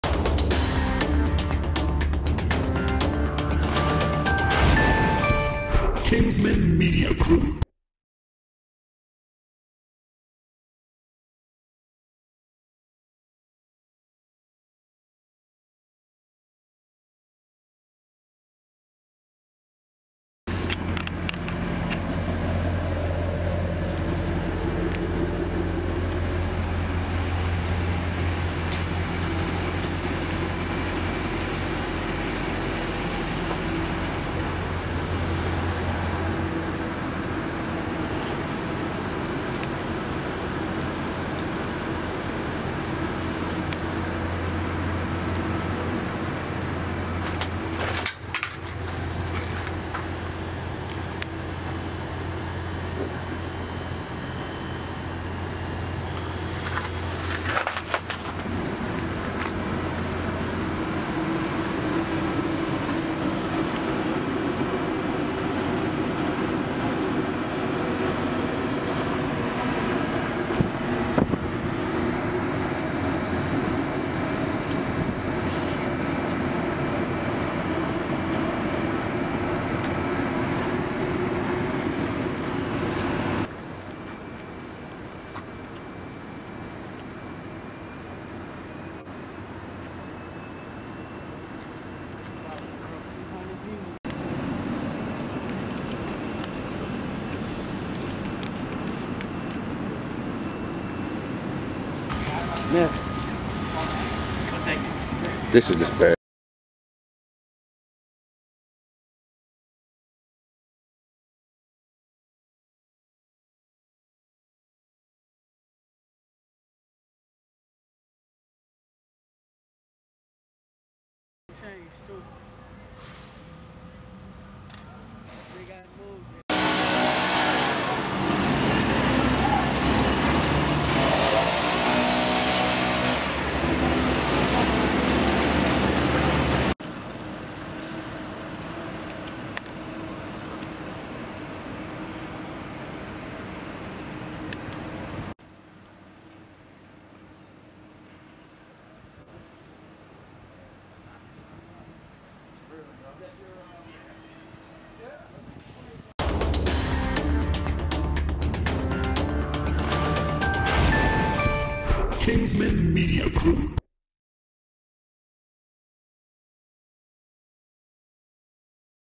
the Aftermath of the city of Atlanta after George Floyd Protest
@KingsmenMediaGroup on the scene in atlanta after George floyd Protest